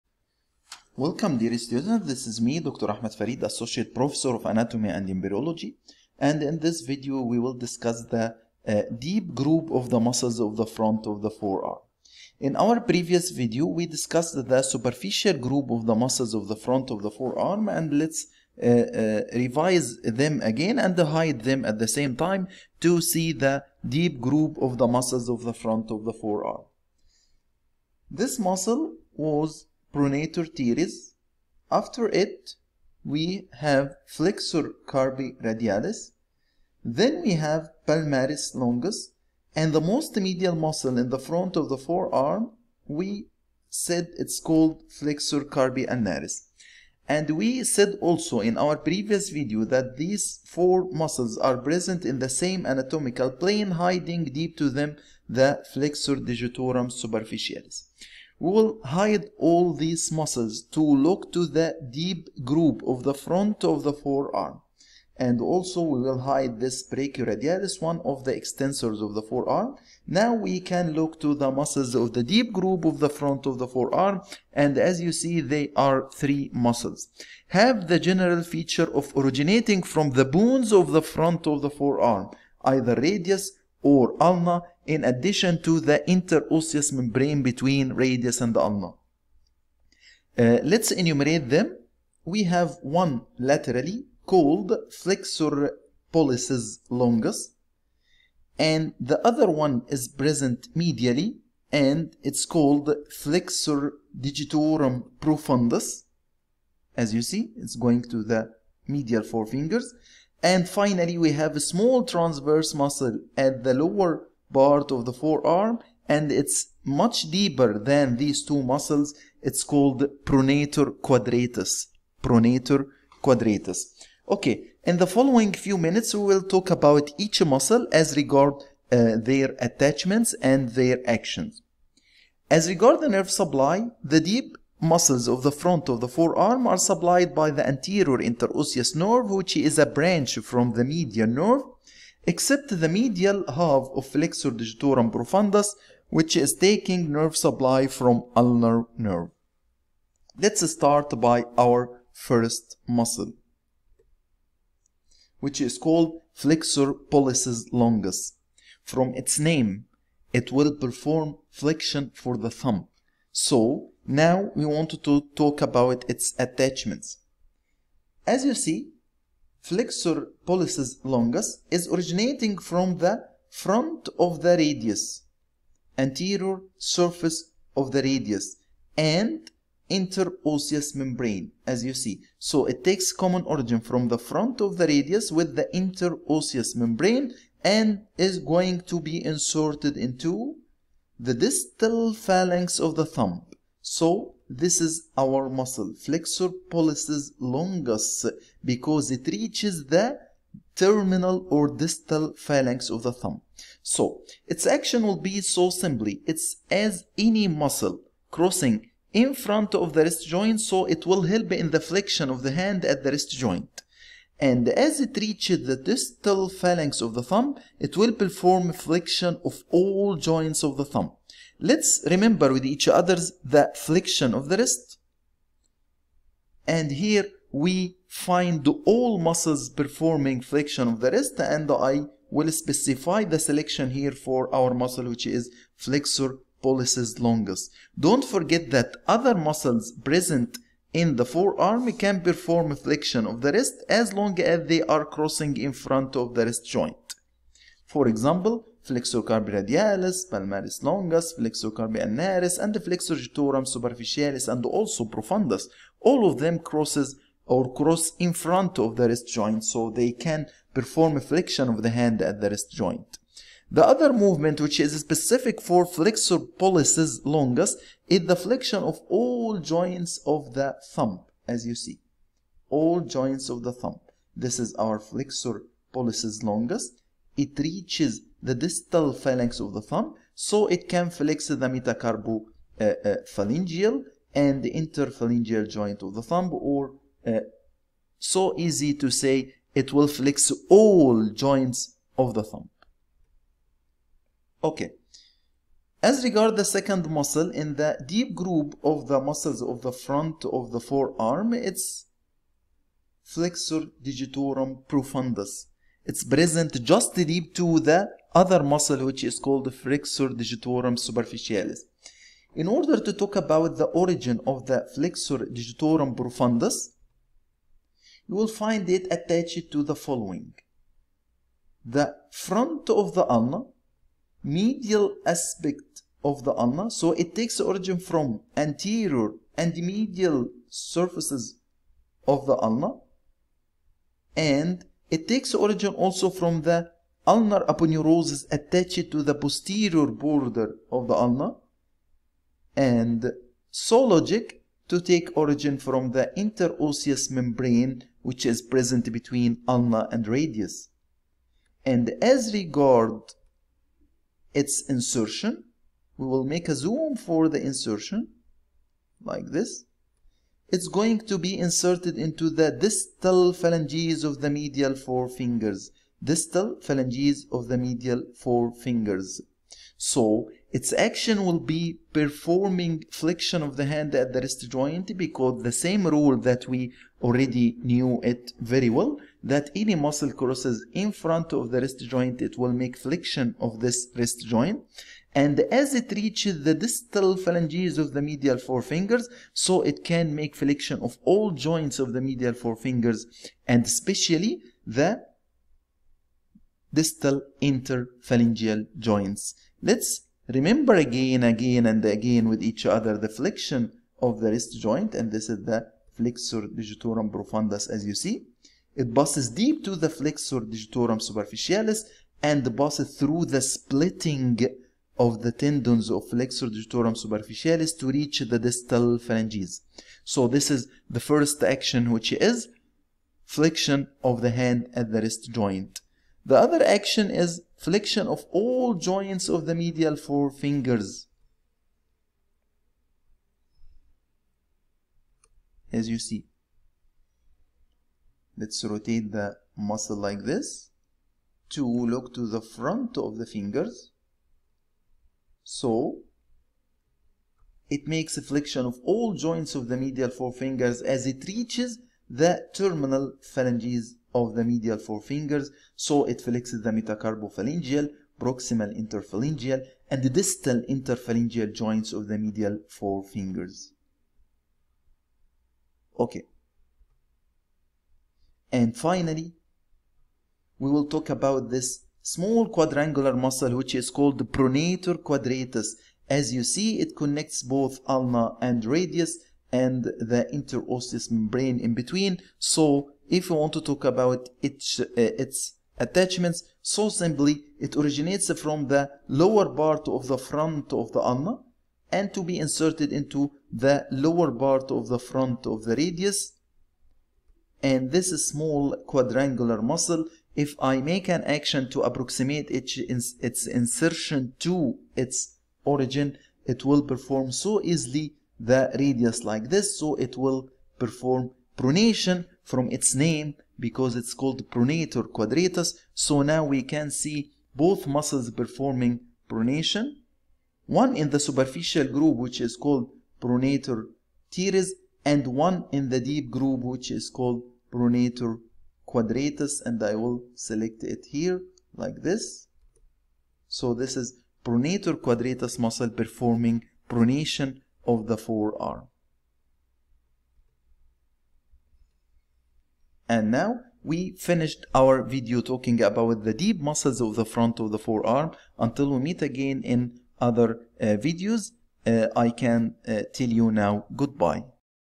Deep Muscles of Front of Forearm (3D) - Dr. Ahmed Farid
3D demonstration of deep group of muscles of front of the forearm as regard their attachments and actions with animations.